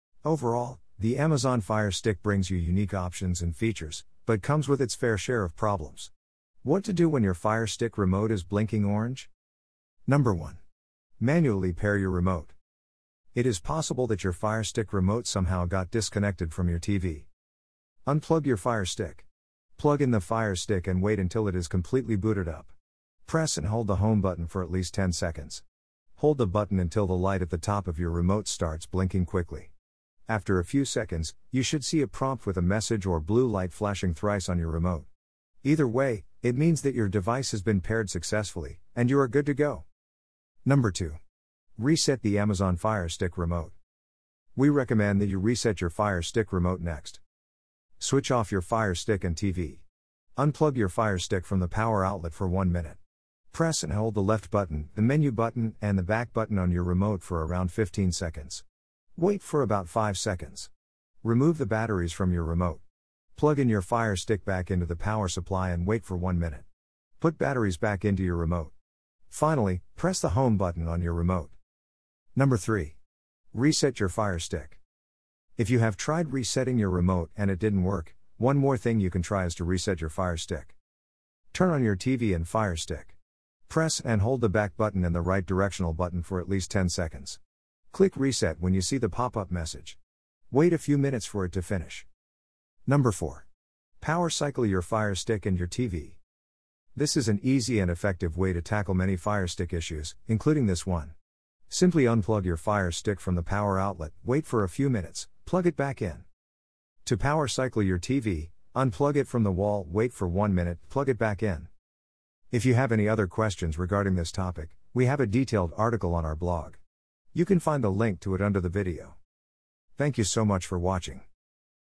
Solved: Fire Stick Remote Blinking Orange Light
Today we will talk about ways to fix your Fire Stick remote when it flashes orange. We will give the most effective methods to tackle the issue.